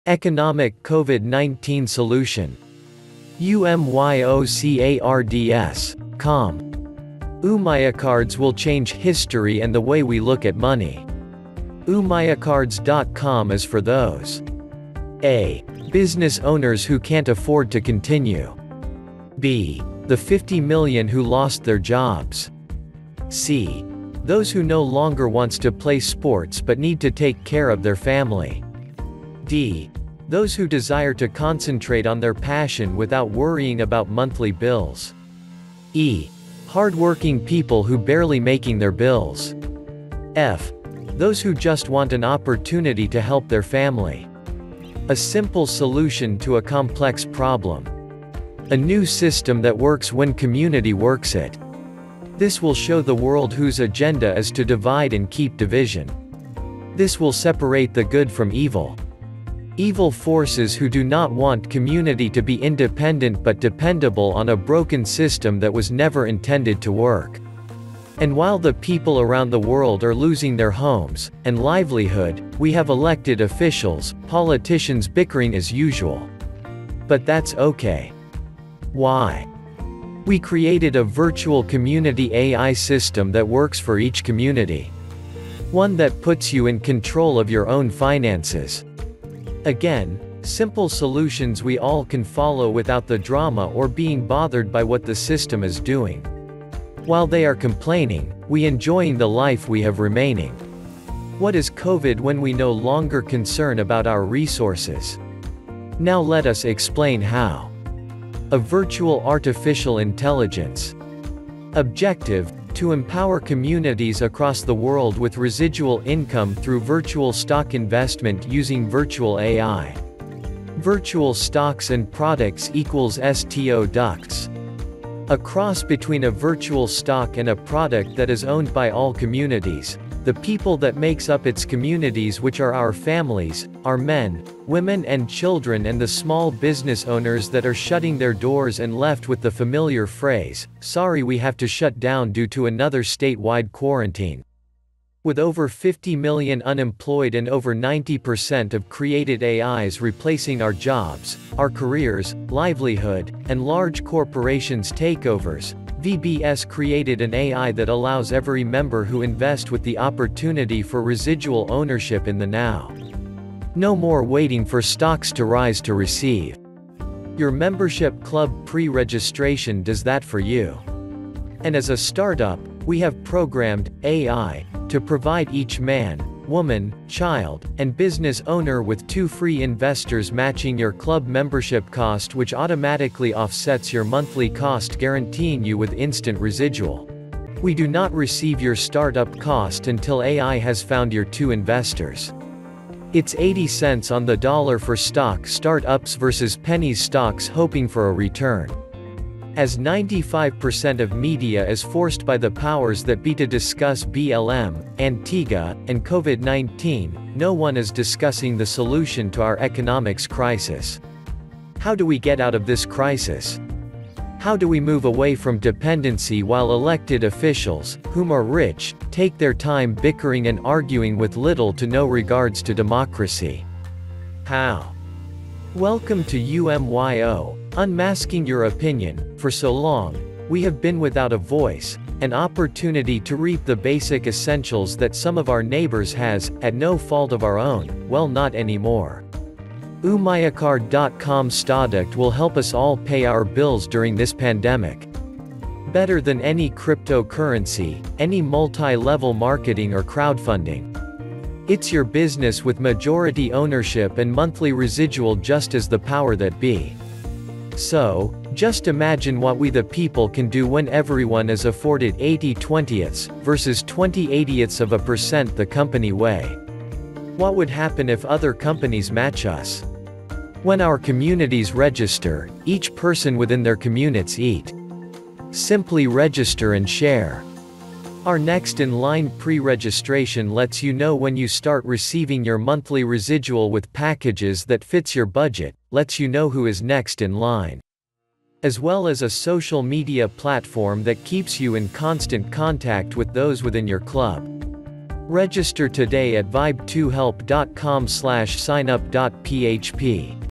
Economic Solution Part 1 Umyocards.com Sto-ducts
ECONOMIC COMMUNITY SOLUTION PART 2

UMYOCARDS.COM

Objective - To empower communities with networking and residual earnings through Virtual Stock Investment; Umyocard.com will change how you earn!

Umyocards.com networking service is for:
a. Business owners facing bankruptcy,
b. The 50 million unemployed,
c. People who put family first, and
d. Anybody who wants to stop worrying about monthly bills.

UMYO = UNMASKING YOUR OPINION
UMYO cards = virtual business presence; Virtual Stocks & Products = STO-DUCTS with
VIRTUAL ARTIFICIAL INTELLIGENCE = AI

UMYO sto-ducts will help us all pay our bills during this pandemic. How? Because the algorithm set in place provides each user with

Your Own Product - Virtual Card
Full ownership with 80% monthly residual going to you
Re-investment opportunities despite limited resources

The UMYO networking system is the answer for our current economic crisis. When everyone in the community becomes a member, no one is left behind. Umyocards.com. is a community/business Virtual Video card social media networking platform.

The UMYO Cards sto-duct is the first cooperatively owned networking stock product of its kind.
A simple solution to a complex problem. A new economic system based on cooperative community networking.

UMYO is better than any crypto currency, multi level marketing, or crowdfunding or go fund me. It's your business with majority ownership and monthly residual.

Our current economic system enforces dependence on wealthy investors over independence and community assets. The result is an accumulation of wealth at the top while the individual and business owner suffers. Uniting in a cooperative community means each participating community rises as a United States, together!

While politicians bickering, people around the world are losing their homes, their jobs and livelihood. Umyocards.com created a Virtual AI system with an algorithm that works for each community and puts YOU in control of your own networking and finances.

UMYO Cards include virtual stock products owned by all communities: families, men, women, children, and the small business owners facing bankruptcy due to COVID 19 quarantines. The AI allows every member to invest in their own individual product with the opportunity for residual ownership. No more waiting for stocks to rise to receive dividends. Your membership club pre-registration fee guarantees monthly earnings from your community.